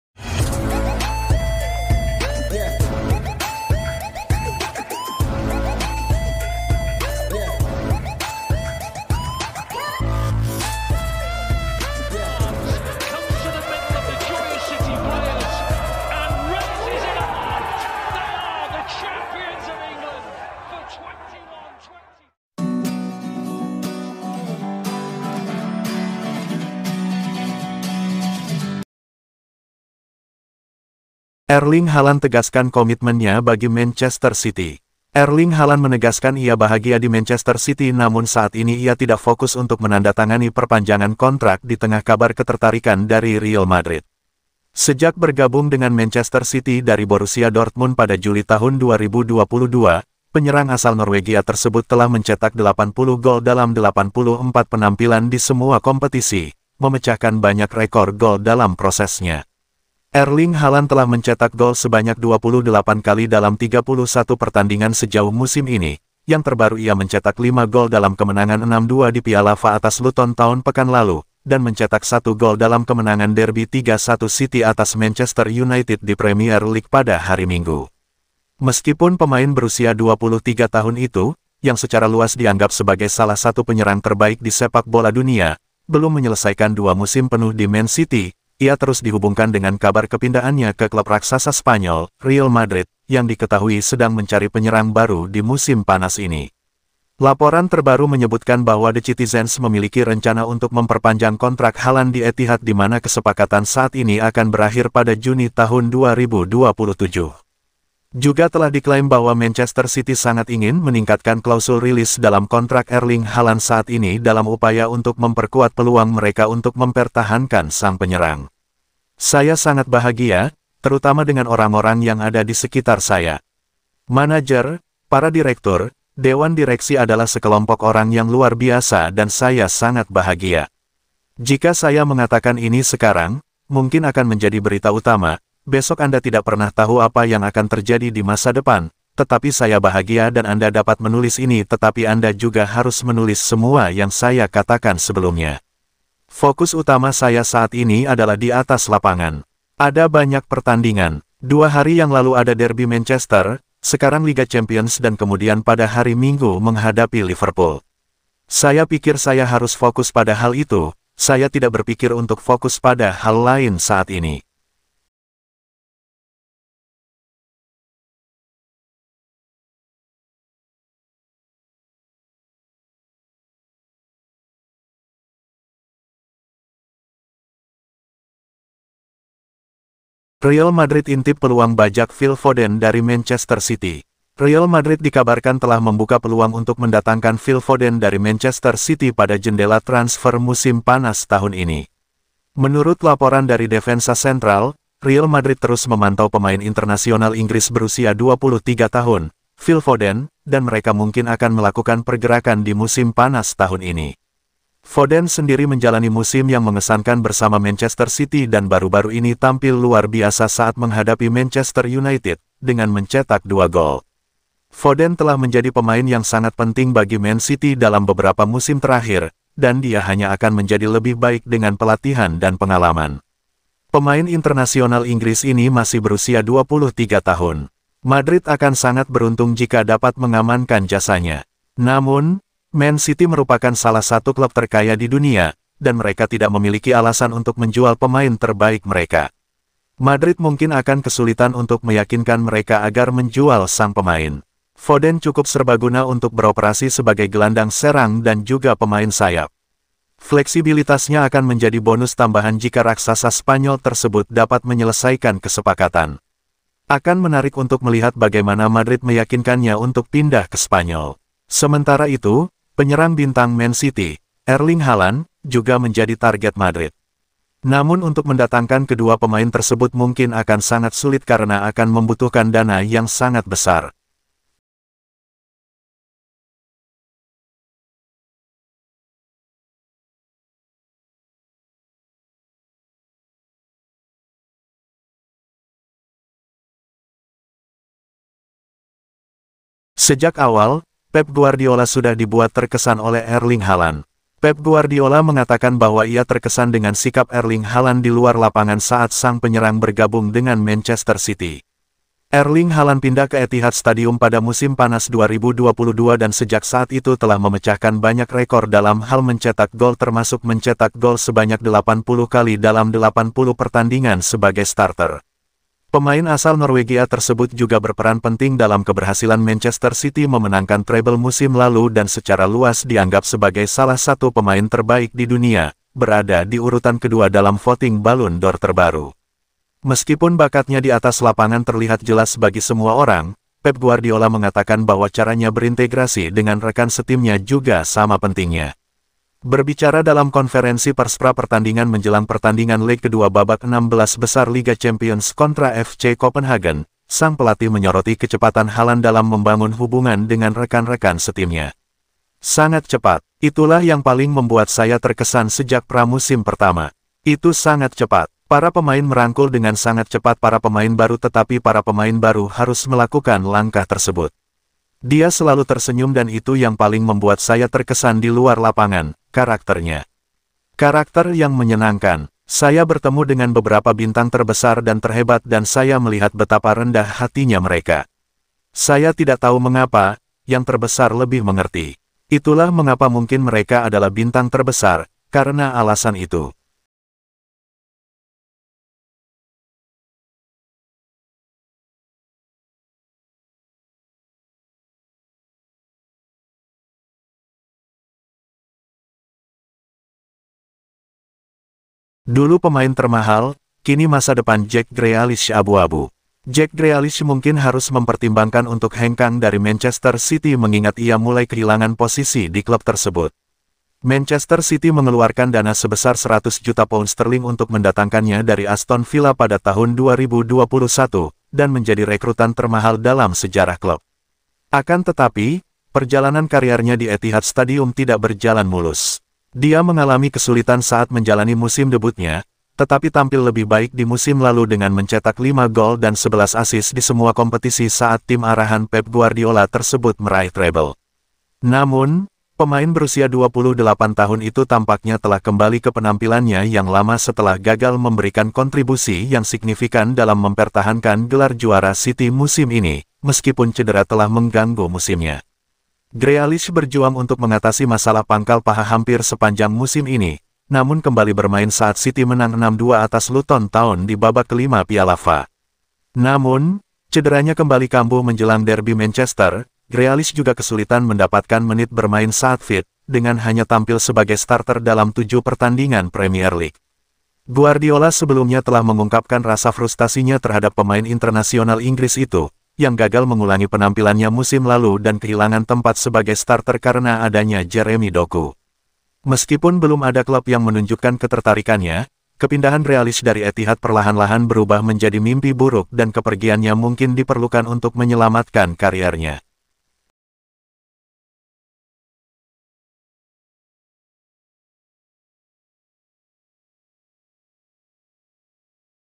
MADRID INCAR FODEN‼️Haaland Pastikan Bertahan📝Musim Terburuk Grealish, Goat Bisa Hengkang🔵
INFO BERITA : MADRID INCAR FODEN‼️Haaland Pastikan Bertahan📝Musim Terburuk Grealish, Goat Bisa Hengkang🔵